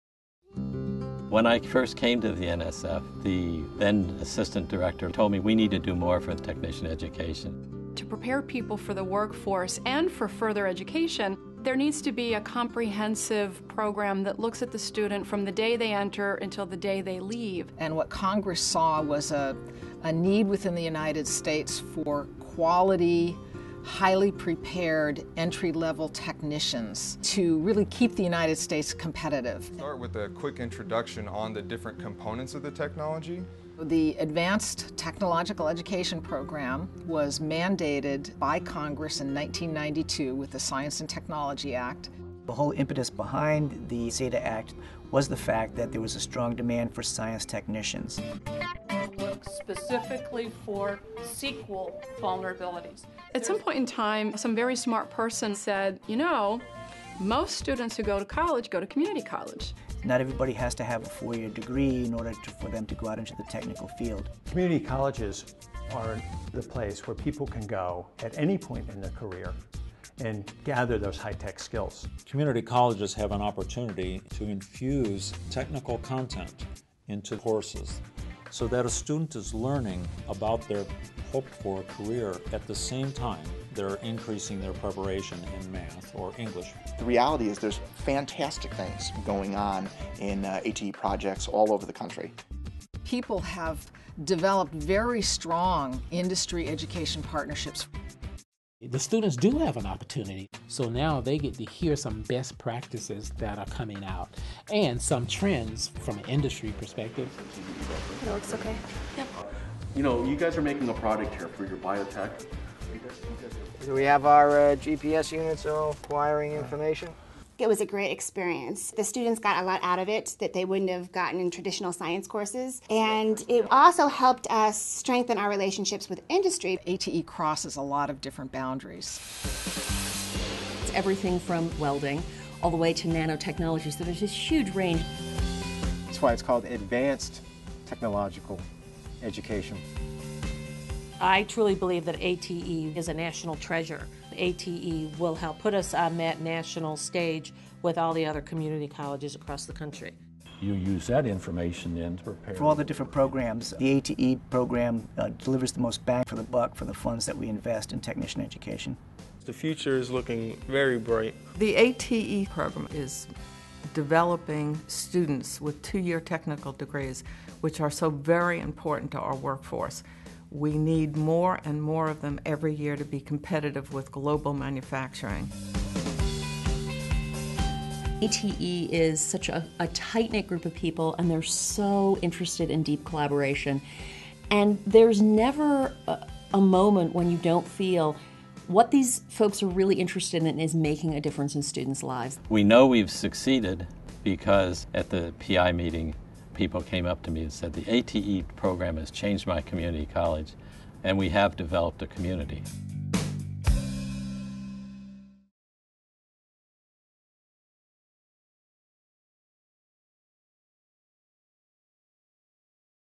ATE@20
The twentieth anniversary provides the perfect context to reflect on the how the ATE program began, what the program has achieved, and where the program might go from here.  Today, science, technology, math and engineering education, as well as our research and development enterprise, remain critical to our nation's competitiveness.  ATE's proven model of success can play an important role in continuing to ensure that we equip current and next generations of science and engineering technicians with the latest skills and tools to compete in the global marketplace.